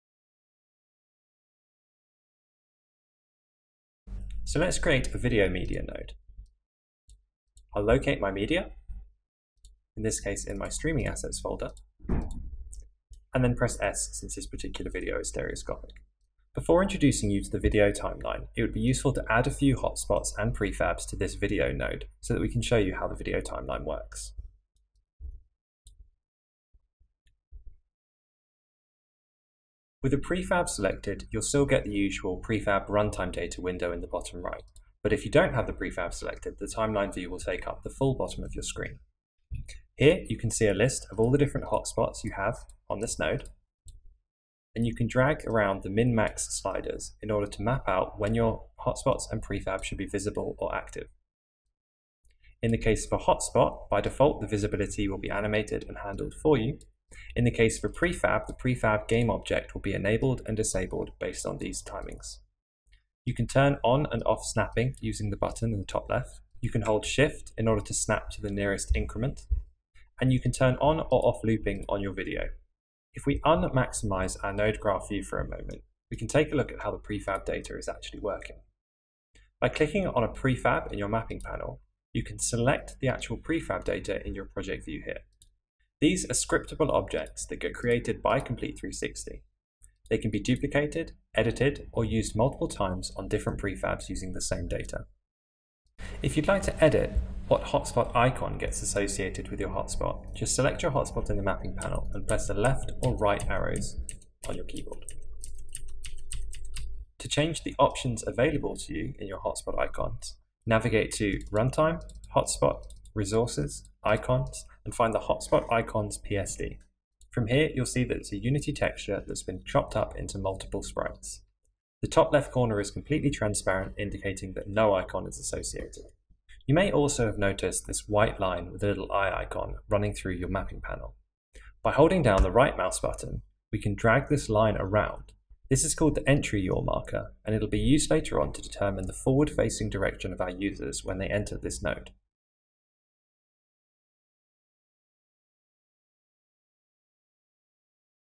DIGITALSALMON | Complete 360 Tour 1.9.0 | The Mapping Panel
DIGITALSALMON presents Complete 360 Tour 1.9.0
The Mapping Panel is a major part of creating your tours. We talk you through creating and manipulating your Hotspots and Prefabs.